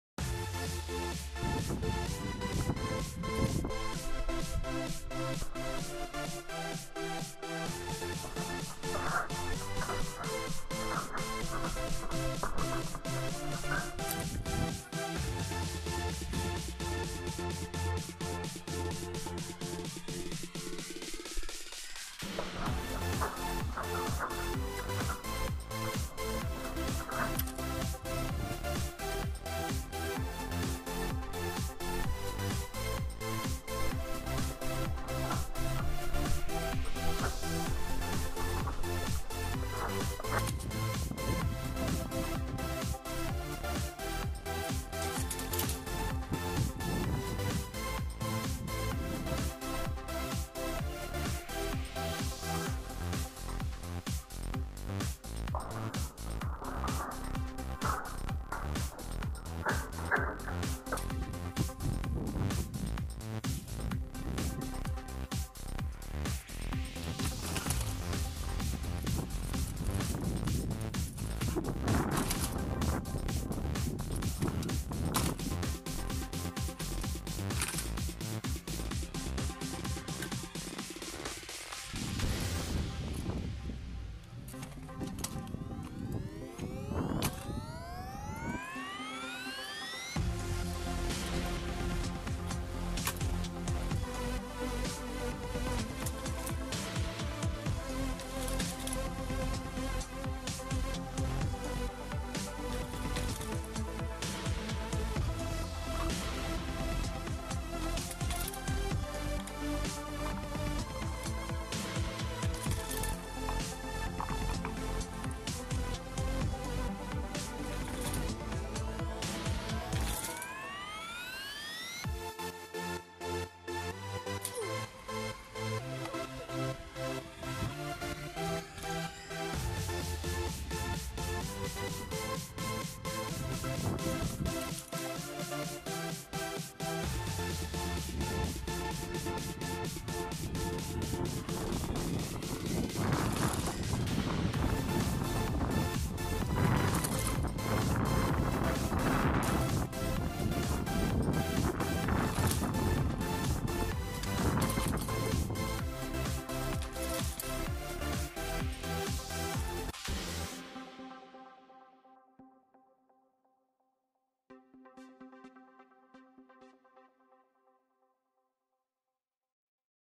Vidéo avec un pot (vtt b'twin)
Vidéo à la place de frameries avec le pot à qui j'ai vendu mon B'twin